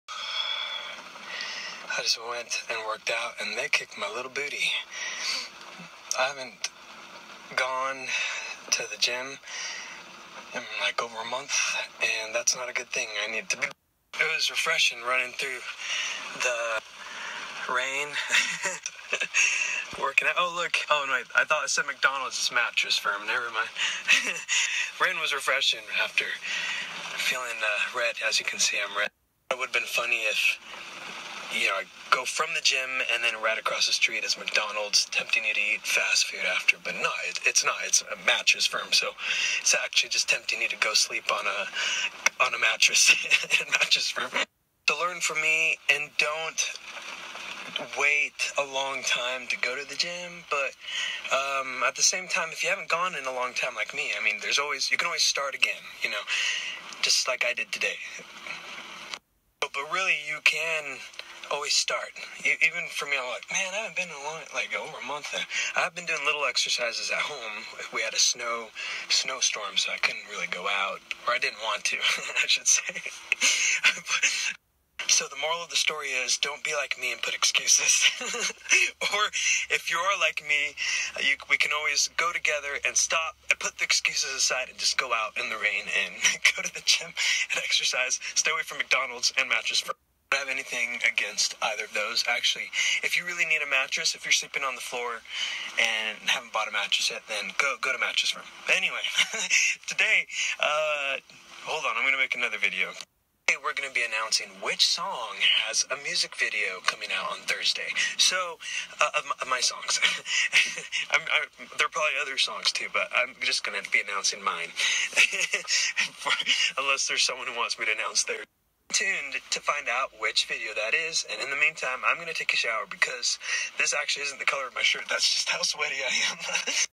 David Archuleta IG stories 01/22/2017
Work out and new vid song title announcement today!!!